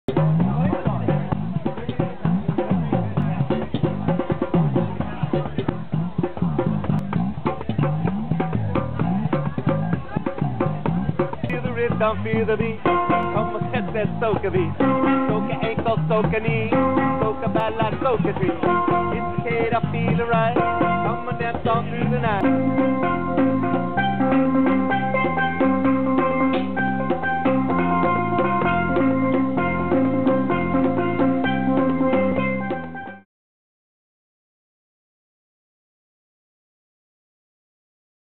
Steel Drum - Steel Parade - Sierra Madre, CA 10/6/07
Steel Parade performing for a Wedding Celebration in Sierra Madre, CA, 10/6/07. visit us